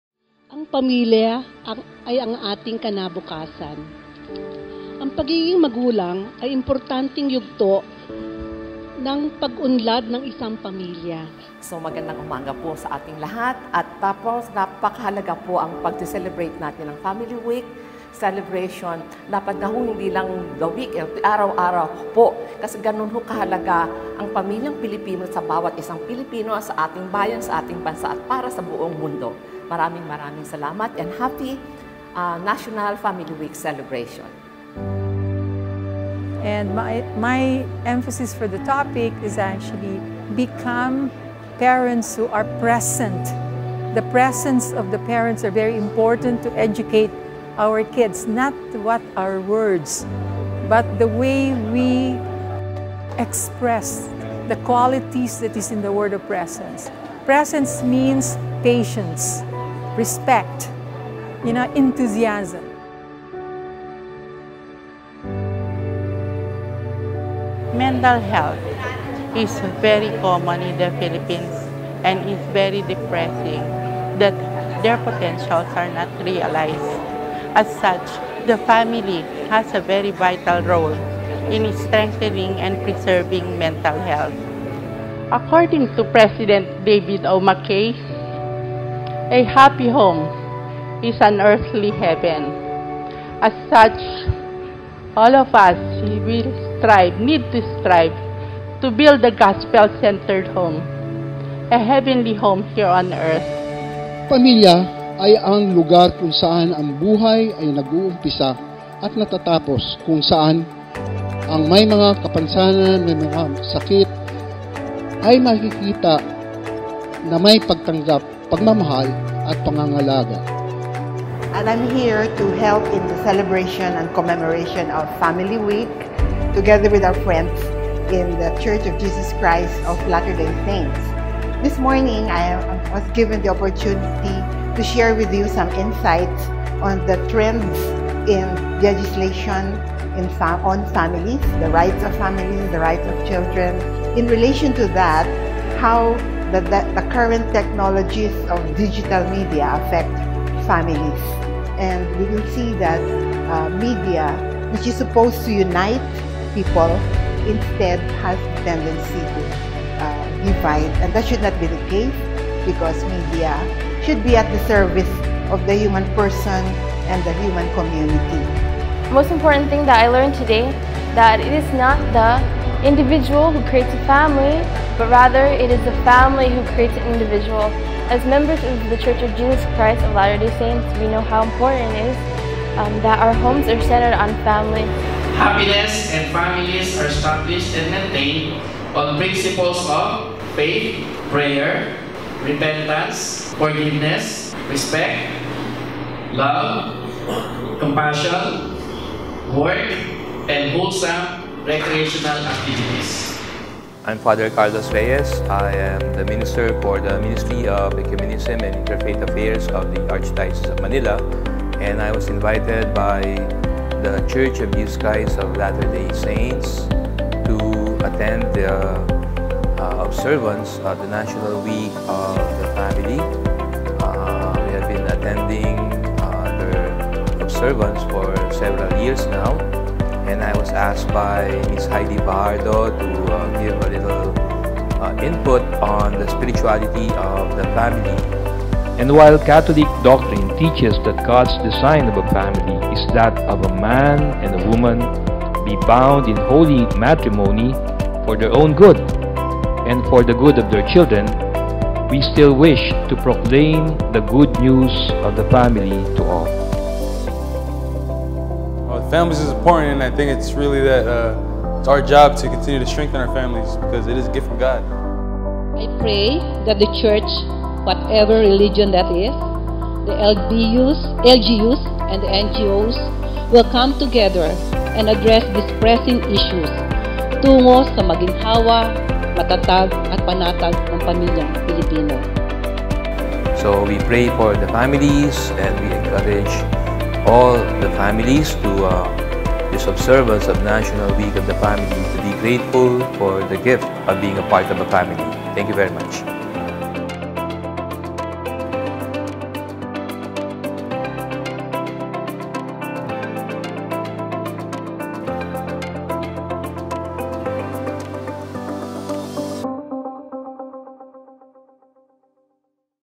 The Church and the Uniharmony Partners Manila Hosts the National Family Week Interfaith Family Forum
In celebration of the 27th National Family Week, The Church of Jesus Christ of Latter-day Saints and its partners, Uniharmony Partners Manila and the National Committee on the Filipino Family, hosted the 2nd Interfaith Family Forum with the theme BFF Building Filipino Families.